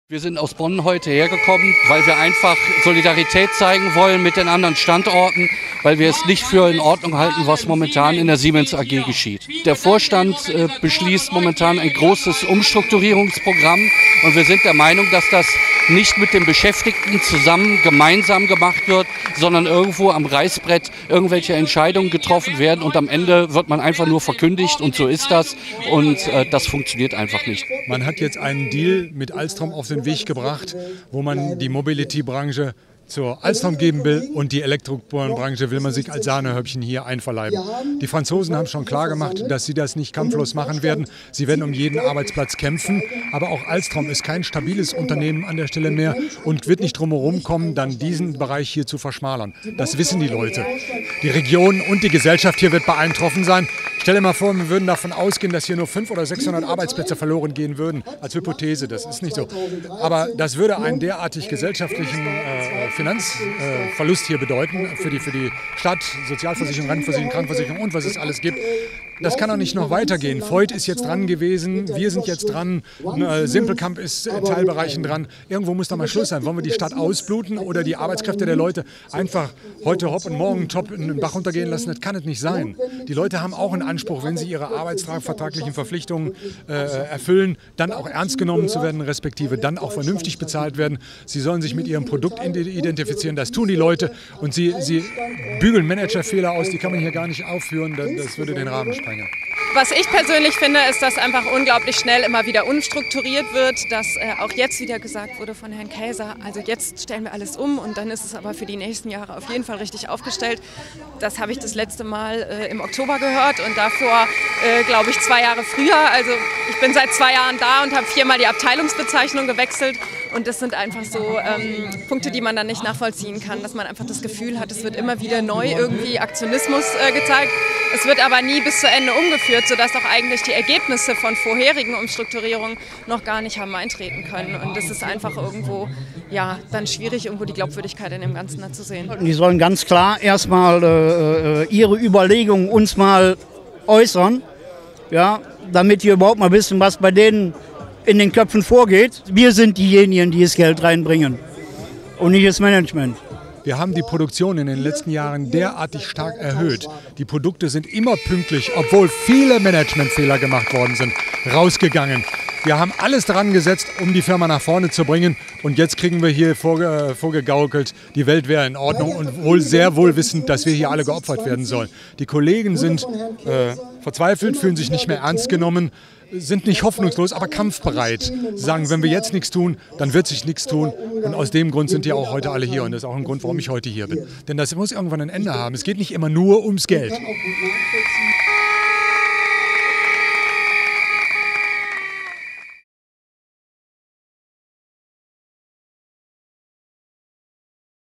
Siemens: 40.000 Jahre Berufserfahrung trotzen Konzernspitze
Die Angst um ihren Arbeitsplatz treibt die Beschäftigten von Siemens auf die Straße. 2000 aus allen 16 Standorten in Nordrhein-Westfalen folgten dem Aufruf der IG Metall zur Protestkundgebung in Krefeld. Sie wehren sich gegen mögliche Abspaltungen von Geschäftsfeldern. Ihr Motto: „One Siemens. Aber mit allen."